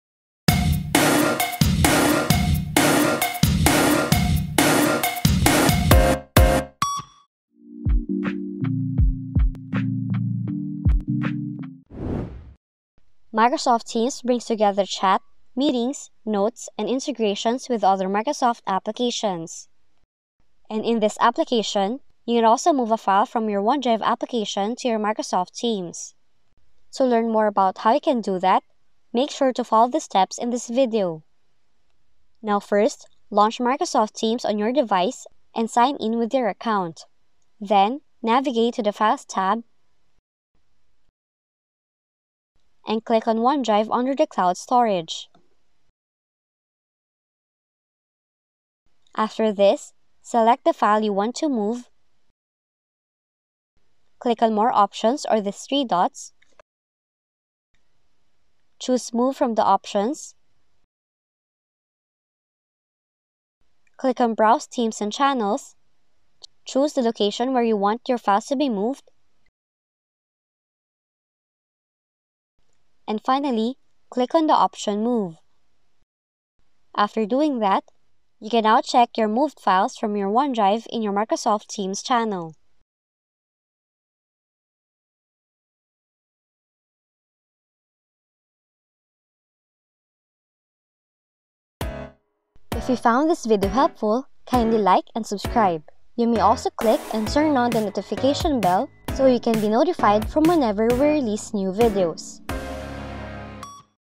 How to Move Files from ONEDRIVE to TEAMS (easily!)
Are you needing to move files from your OneDrive account over to your Teams account?  If so, you can move them quite easily once you know how.  Watch this video tutorial to see the step-by-step!